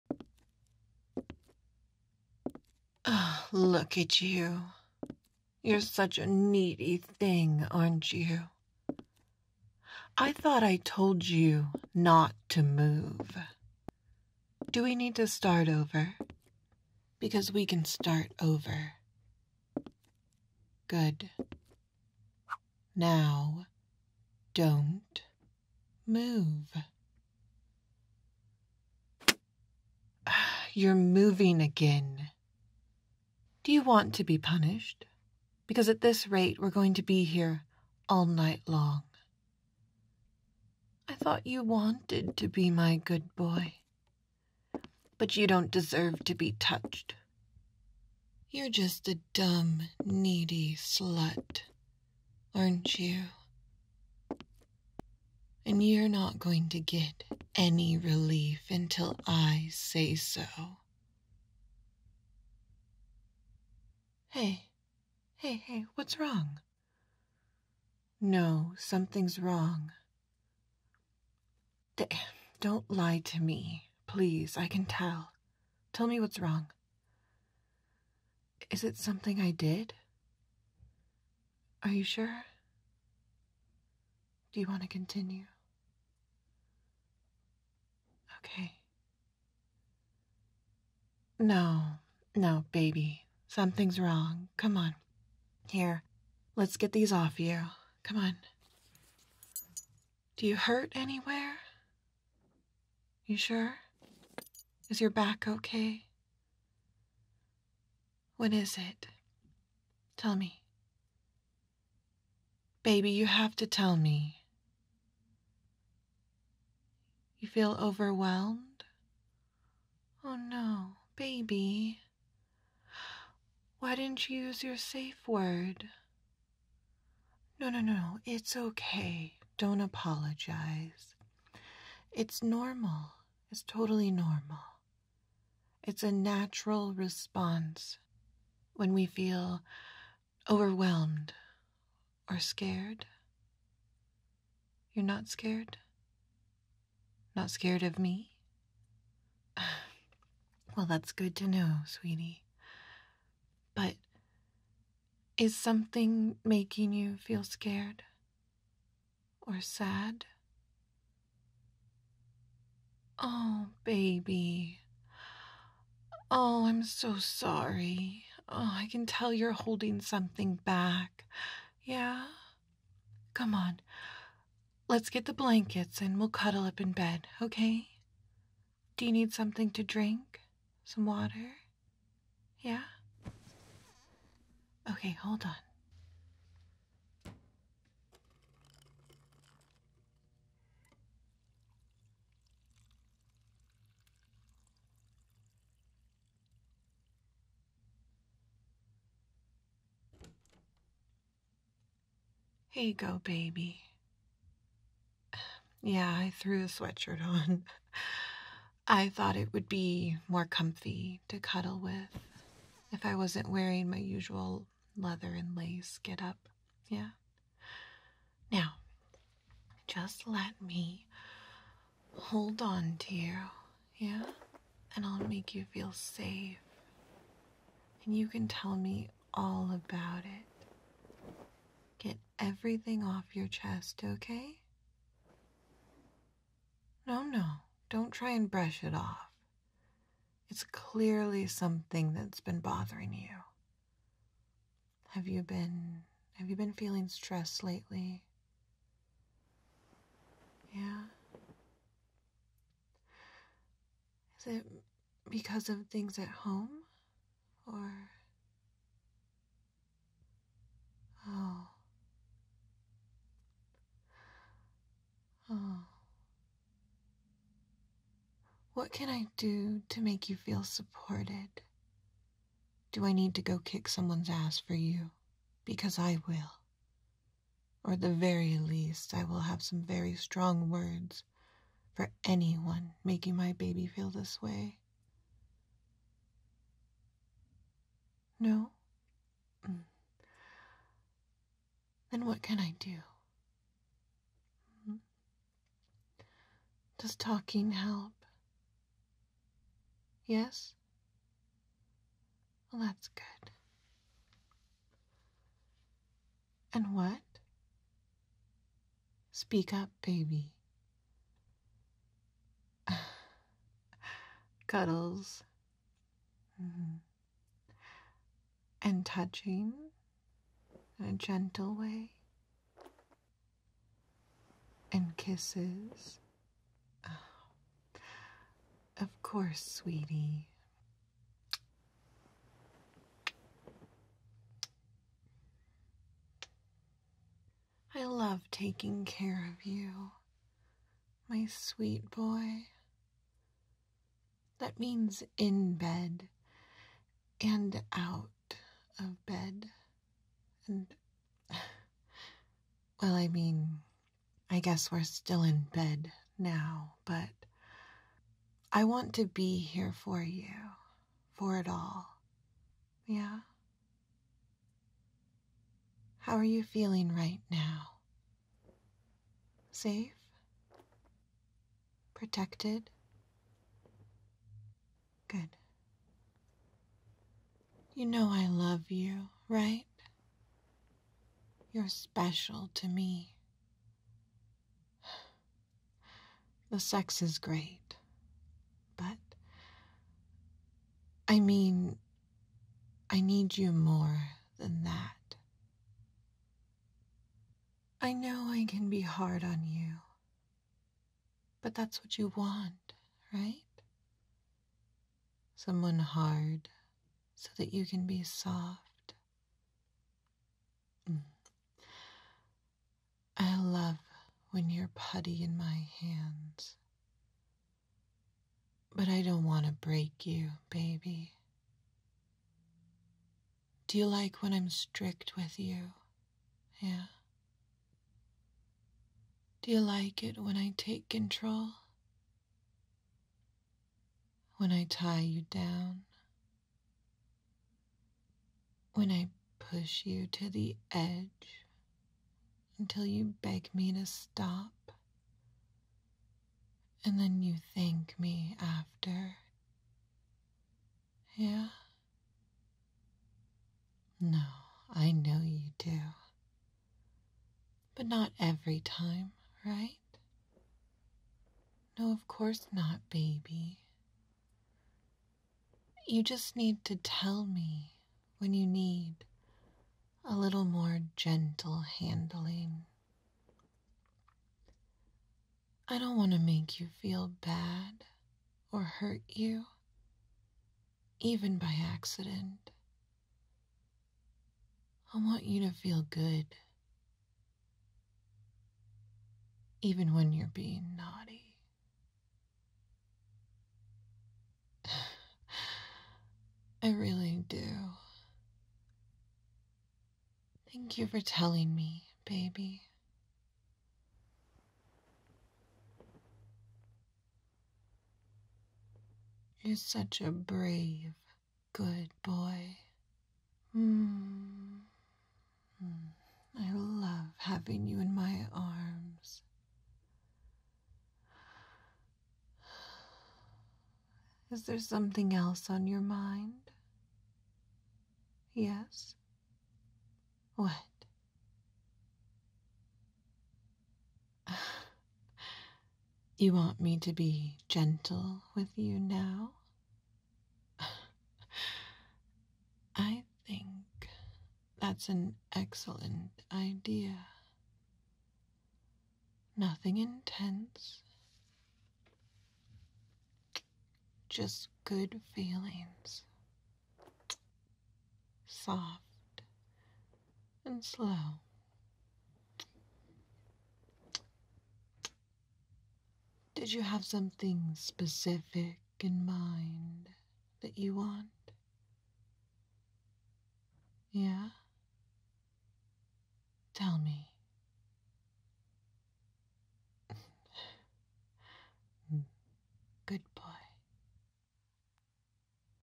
F4M Mommy Domme Girlfriend Gentle Aftercare | Degradation | Praise | One Spank | Good Boy | ASMR
Good boy.

🖤 Click for more goodies! 🖤

Your girlfriend notices a little distant and distracted while playing and she stops to check in with you and offer some advice, cuddles, and praise.

Written & Recorded by GingerWitch
Editing & Mastering by GingerWitch
All artwork, effects and music used with permission and proper licensing.

Thank you for listening to my work!
💖Ginger